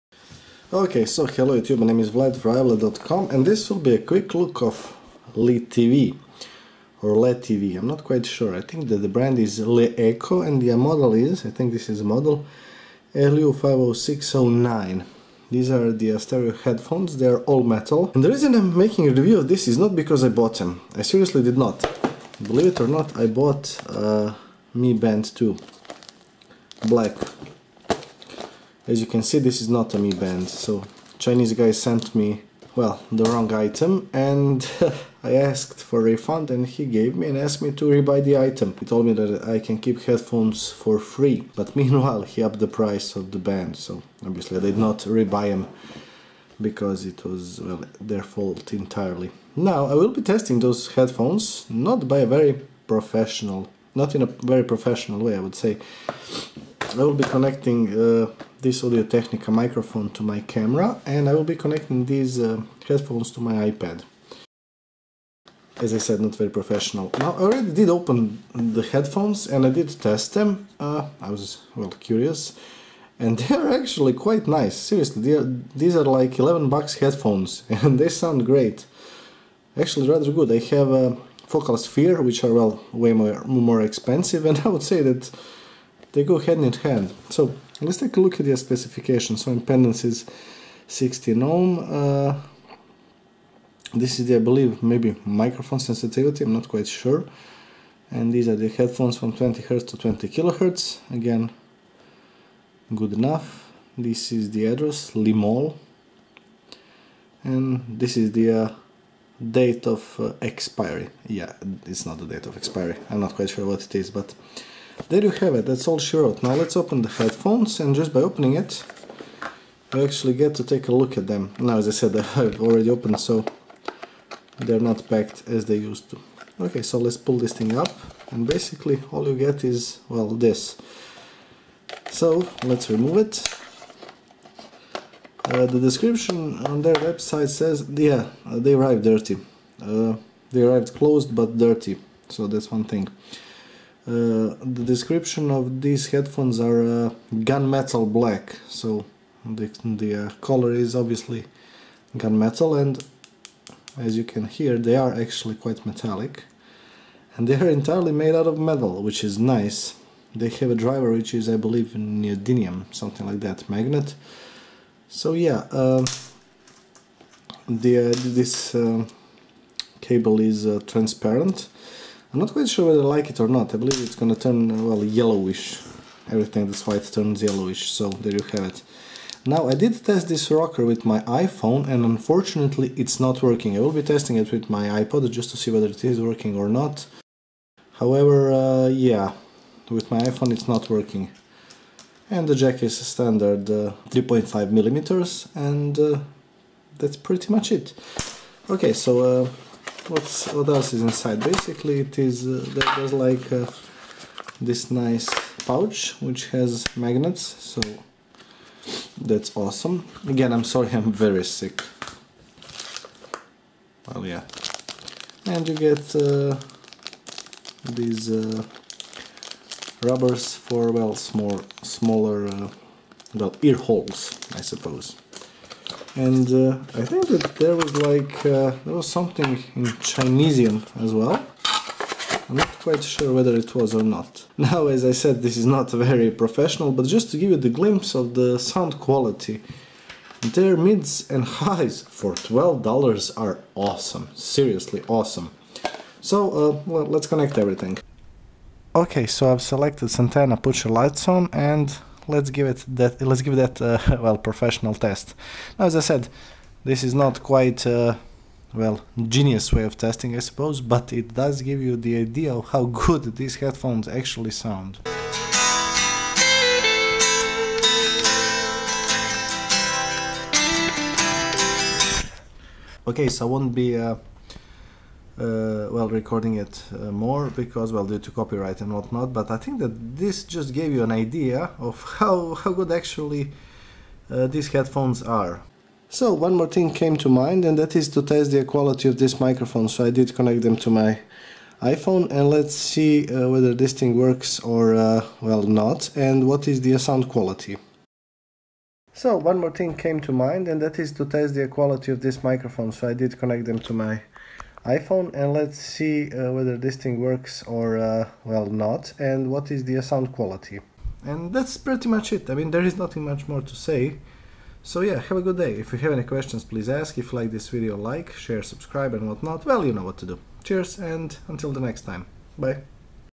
Quick look at LeTV full metal in ear headphones
TLDR; best 12$ not spent, but seriously, worth 12$ even if they fall apart.

Good
- Cheap
- Awesome sound
- (heavy) metal
- volume rocker, good quality microphone.

Bad:
- For 12$ - nothing really, worth every $ (IMHO)

Specs:
Phones - 20-20KHz
Impendance: 16 OHM
Mic - 96dB +/-3dB

if you find this useful, you are rich as F**K and want to pay me a beer:

BCN-ME: 1P7drxDQALyJDm8rCVSuhEXkiS5LnYFpRr

ETH-ME 0x21E4cC6BEf890000B1cD52e23F426382d5651B92